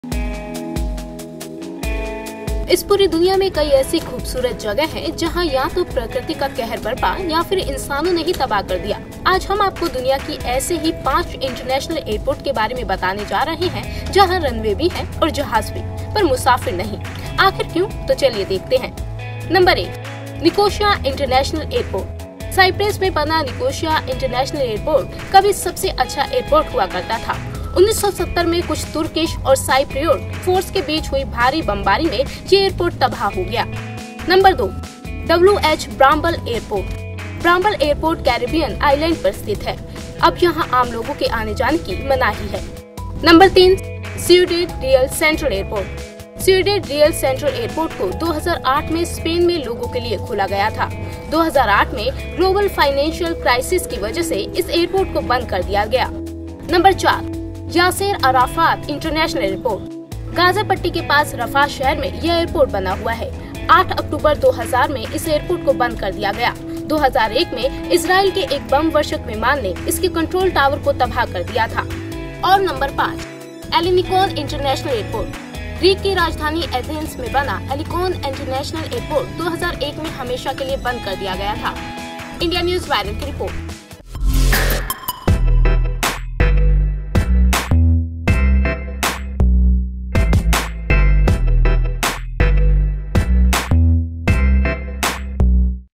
दुनिया के 5 ऐसे एयरपोर्ट जहां रनवे और जहाज हैं पर मुसाफिर नहीं, आखिर क्यों ? INDIA NEWS VIRAL
दुनिया के 5 ऐसे एयरपोर्ट जहां रनवे और जहाज हैं पर मुसाफिर नहीं, आखिर क्यों ? देखें रिपोर्ट